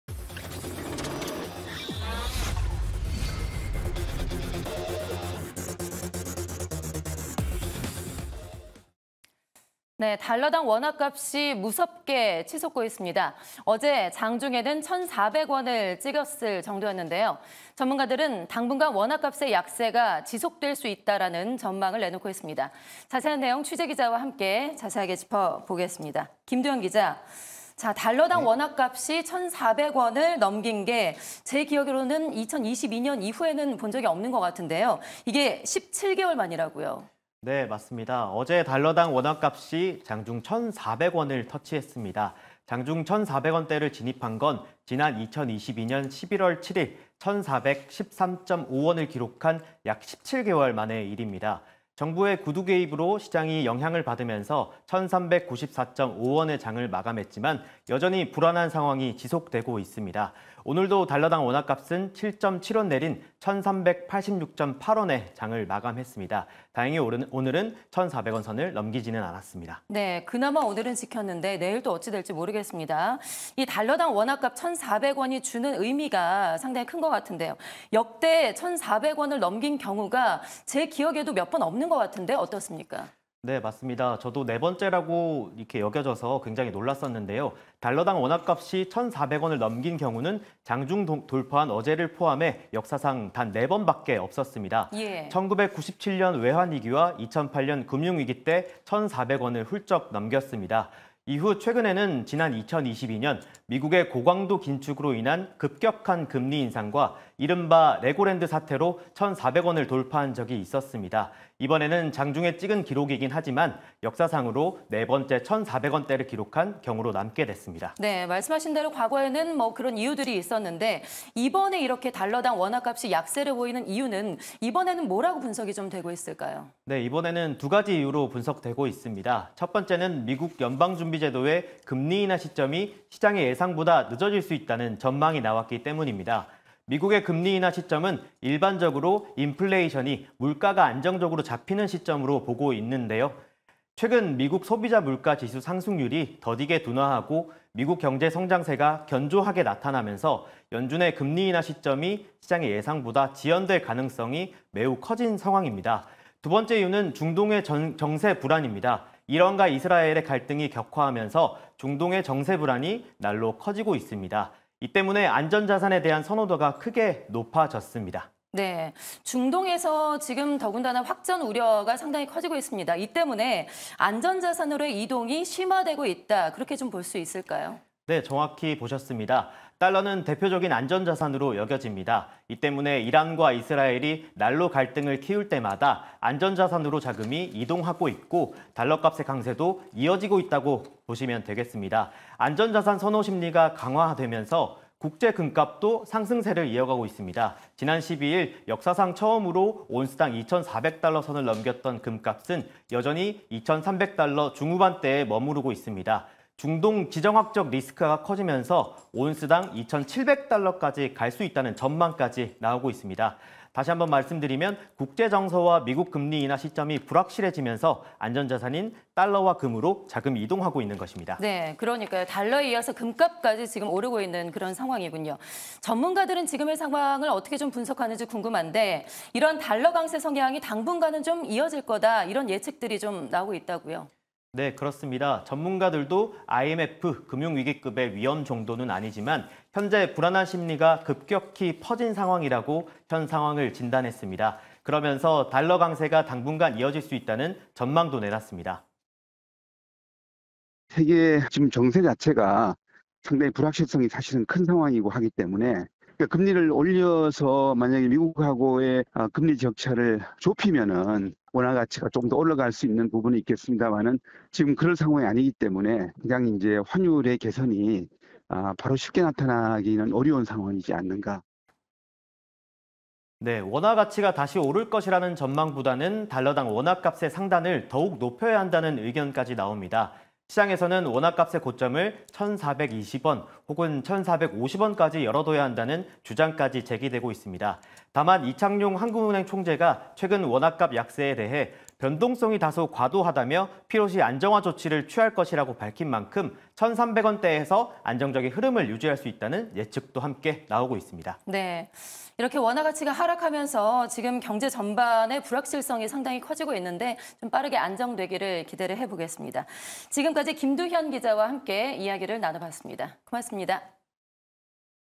[집중취재] 달러당 원화값 1400원 깨졌다…외환위기급 폭락
【 앵커멘트 】
  달러당 원화값이 무섭게 치솟고 있습니다.
  어제 장중에는 1400원을 찍었을 정도인데요.
  전문가들은 당분간 원화값의 약세가 지속될 수 있다는 전망을 내놓고 있습니다.
  자세한 내용 스튜디오 나와 있는 보도국 취재기자와 이야기 나눠보겠습니다.
  김두현 기자, 안녕하세요.

【 앵커멘트 】
  달러당 원화값이 1400원을 넘긴 건 17개월 만이라고요?

  어제 달러당 원화값이 장중 1400원을 터치했습니다.

  장중 1400원대에 진입한 건 지난 2022년 11월 7일 1413.5원을 기록한 이후 약 17개월 만입니다.

  정부의 구두개입으로 시장이 영향을 받으면서 1394.5원에 장을 마감했지만, 여전히 불안한 상황이 지속되고 있습니다.

  오늘도 달러당 원화값은 7.7원 내린 1386.8원에 장을 마감했습니다.

  다행히 오늘은 1400원 선을 넘기지는 않았습니다.

【 앵커멘트 】
  달러당 원화값 1400원이 주는 의미가 굉장히 큰 것 같은데요.
  역대 1400원을 넘긴 경우가 굉장히 드물다고요?

【 기자 】
  네, 맞습니다.

  달러당 원화값이 1400원을 넘긴 경우는 장중 돌파한 어제를 포함해 역사상 4번밖에 없었습니다.

  1997년 외환위기와 2008년 금융위기 때 1400원을 훌쩍 넘겼습니다.

  이후 최근에는 지난 2022년 미국의 고강도 긴축으로 인한 급격한 금리인상과 이른바 레고랜드 사태로 1400원을 돌파한 적이 있었습니다.

  이번에는 장중에 찍은 기록이긴하지만, 역사상으로 4번째 1400원대를 기록한 경우로 남게됐습니다.

【 앵커멘트 】
  그렇군요. 그럼 이번 달러당 원화값 약세, 그 이유는 무엇인가요?

【 기자 】
  두 가지 이유로 분석됩니다.

  첫 번째는 미국 연방준비제도의 금리 인하 시점이 시장의 예상보다 늦어질 수 있다는 전망이 나왔기 때문입니다.

  미국의 금리 인하 시점은 일반적으로 인플레이션이 안정적으로 잡히는 시점으로 보고 있는데요.

  최근 미국 소비자물가지수 상승률이 더디게 둔화하고, 미국 경제 성장세가 견조하게 나타나면서 연준의 금리 인하 시점이 시장의 예상보다 지연될 가능성이 매우 커진 상황입니다.

  두 번째 이유는 중동의 정세 불안입니다.

  이란과 이스라엘의 갈등이 격화하면서 중동의 정세 불안이 날로 커지고 있습니다.

  이 때문에 안전자산에 대한 선호도가 크게 높아졌습니다.

【 앵커멘트 】
   중동에서의 확전 우려가 커지면서 안전자산으로의 이동이 심화되고 있다고 봐도 되는 건가요?

  달러는 대표적인 안전자산으로 여겨집니다.

  이 때문에 이란과 이스라엘이 날로 갈등을 키울 때마다 안전자산으로 자금이 이동하고 있고, 달러값의 강세도 이어지고 있다고 보시면 되겠습니다.

  안전 자산 선호 심리가 강화되면서 국제 금값도 상승세를 이어가고 있습니다.

  지난 12일 역사상 처음으로 온스당 2천400달러 선을 넘어섰던 금값은 여전히 2천300달러 중후반대에 머무르고 있는데요.

  중동 지정학적 리스크가 커지면서 온스당 2천700달러까지 갈 수 있다는 전망까지 나오고 있습니다.

  즉, 국제 정서와 미국의 금리 인하 시점이 불확실해 지면서 안전자산인 달러와 금으로 자금이 이동하고 있는 것입니다.

【 앵커멘트 】
  달러와 금값의 강세가 연일 이어지고 있네요.
  전문가들은 이러한 달러 강세 성향이 당분간 이어질 거라는 예측하고 있다고요?

  전문가들도 IMF, 금융위기급의 위험정도는 아니지만, 현재 불안한 심리가 급격히 퍼진 상황이라고 현 상황을 진단했습니다.

  그러면서 달러 강세도 당분간 이어질 수 있다는 전망을 내놨습니다.

  ▶ 인터뷰(☎) : 이정희 / 중앙대 경제학과 교수
- "세계 정세 자체가 상당히 불확실성이 큰 상황이고, (우리나라가) 금리를 올려서 미국과의 금리 격차를 좁히면 원화 가치가 조금 올라갈 수 있겠지만, 그럴 상황이 아니기 때문에 당장은 환율의 개선이 쉽게 나타나기는 어려운 상황…"

  원화 가치가 다시 오를 것이라는 전망보다는 달러당 원화값의 상단을 더욱 높여야 한다는 의견까지 나옵니다.

  시장에서는 원화값의 고점을 1420원 혹은 1450원까지 열어둬야 한다는 주장까지 제기되고 있습니다.

  다만, 이창용 한국은행 총재가 최근 원화값 약세에 대해 "변동성이 다소 과도하다"며 "필요시 안정화 조치에 취할 것"이라고 밝힌 만큼, 1300원 대에서 안정적인 흐름을 당분간 유지할 수 있다는 예측도 나오고 있습니다.

  【 앵커멘트 】
  원화 가치가 하락을 하면서 경제 전반의 불확실성이 커지고 있는데요.
  빠르게 안정화됐으면 하는 바람입니다.
  이상, 김두현 기자와 이야기 나눠봤습니다.